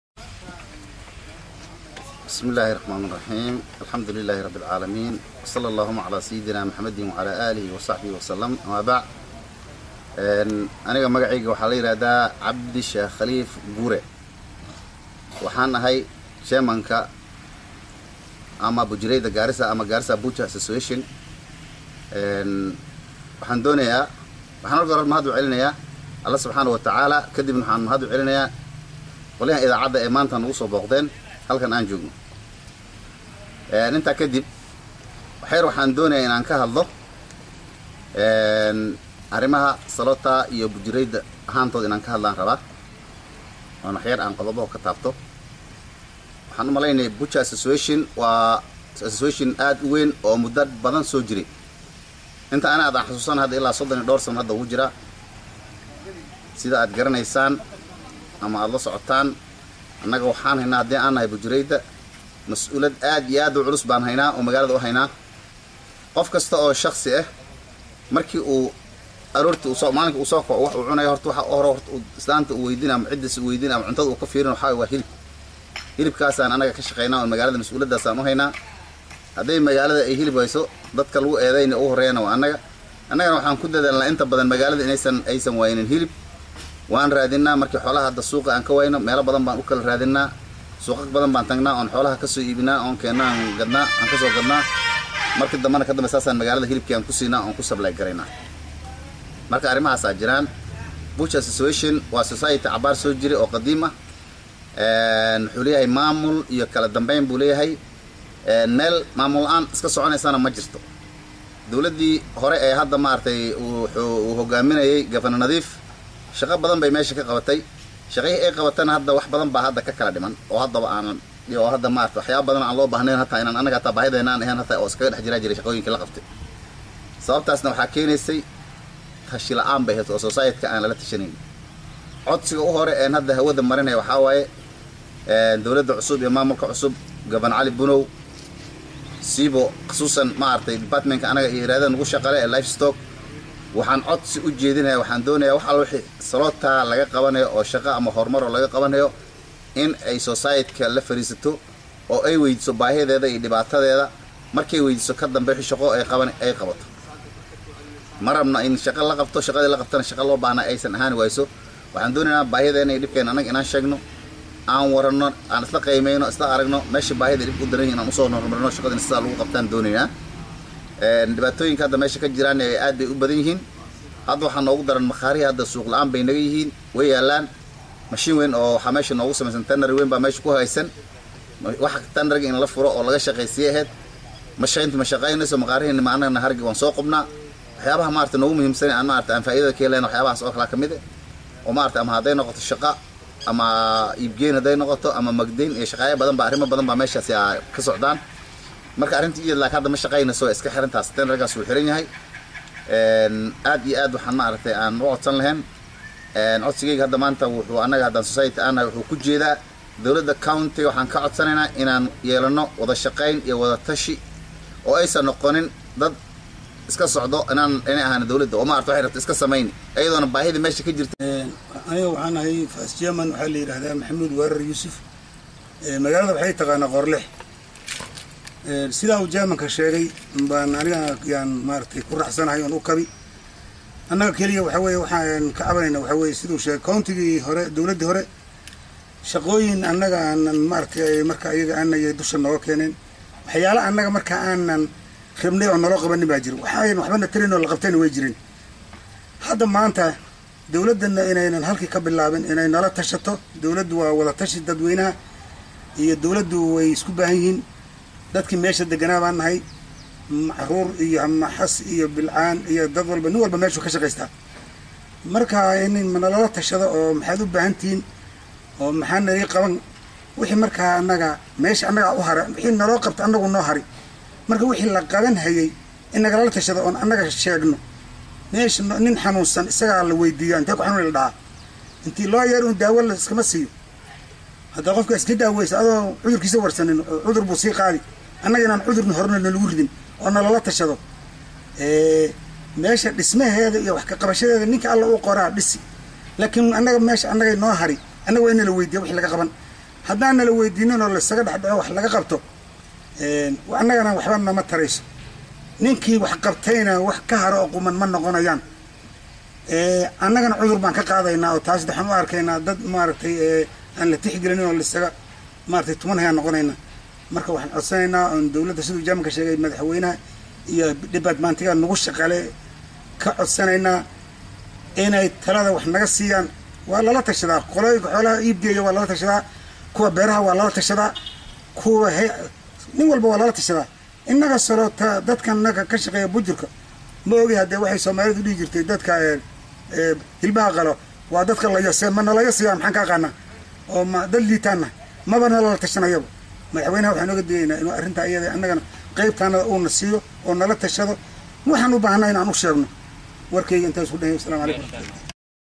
Gudiga Buujarayda Ama Salootada Magaalada Garissa Oo Baaq Kulul Soo Saareen
HANALA TASHTO HADII KALE SIDII NADIIF CAMAL WAXBA MA HAGAAGAYO